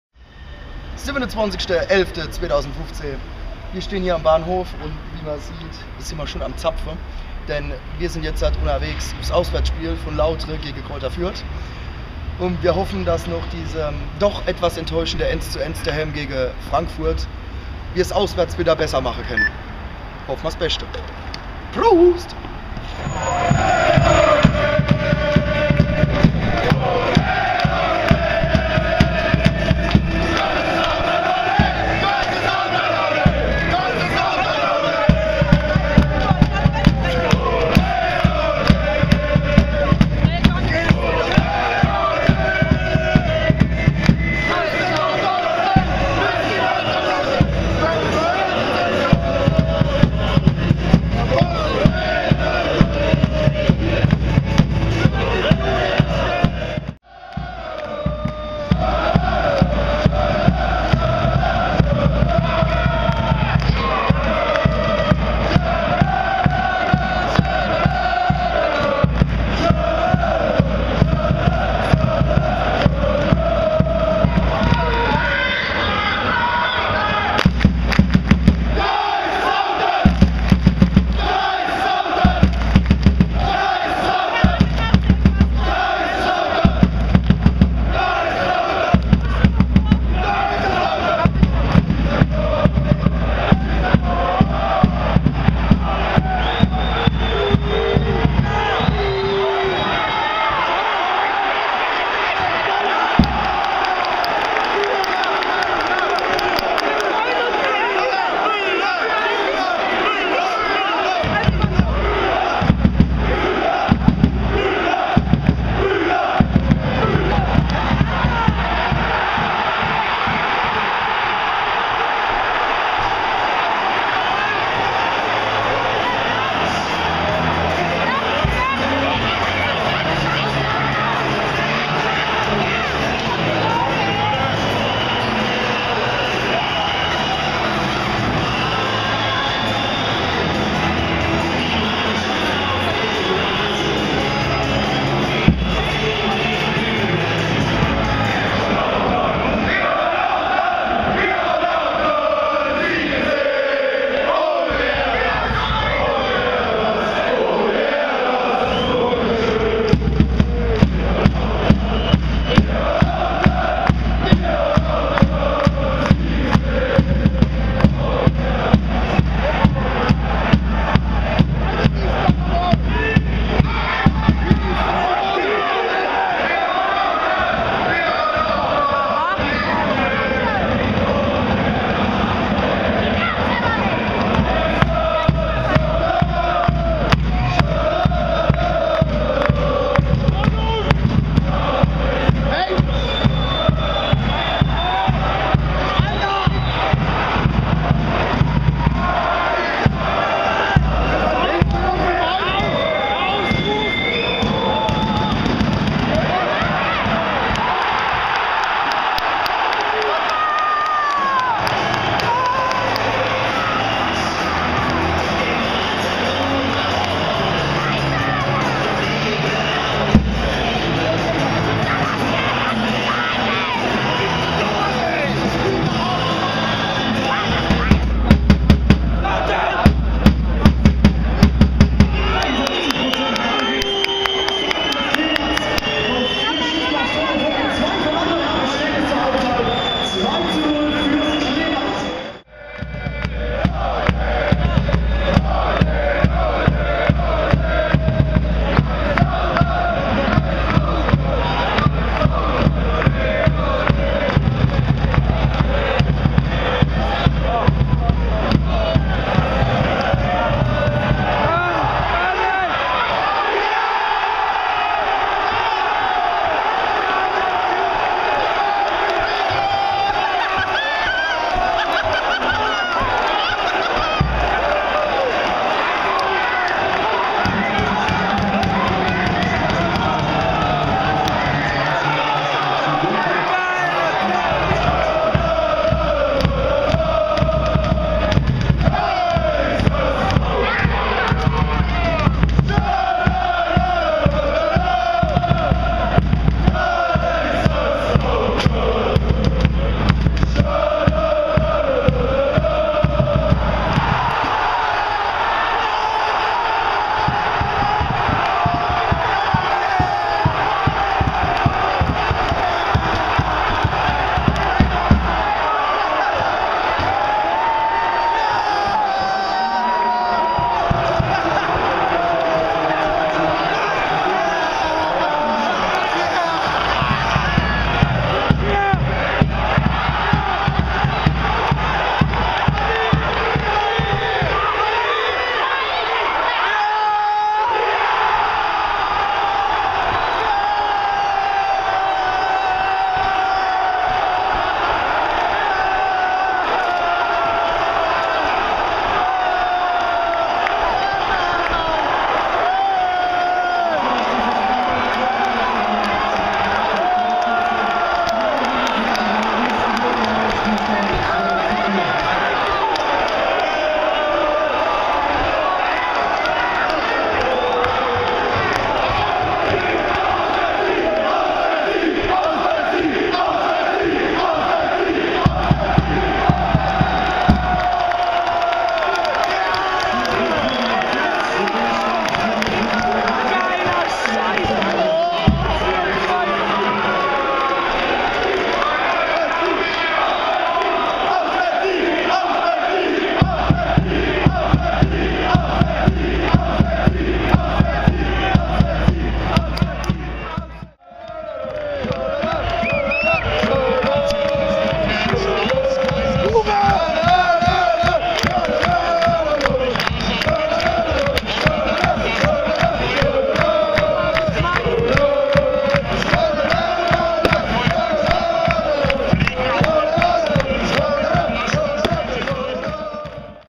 SpVgg Greuther Fürth 2:4 1. FC Kaiserslautern - 27.11.2015 - So kann´s immer laufen!
Hi zusammen!

WAS EIN SPIEL!!! Nach einem 2:0 Rückstand hat unser FCK es geschafft die Partie komplett zu drehen und die 3 Phnkte aus Bayern in die Pfalz zu holen!
Was will man als Fan mehr?

Über Feedback, Kommentare, Bewertungen oder Abos würden wir uns sehr freuen :)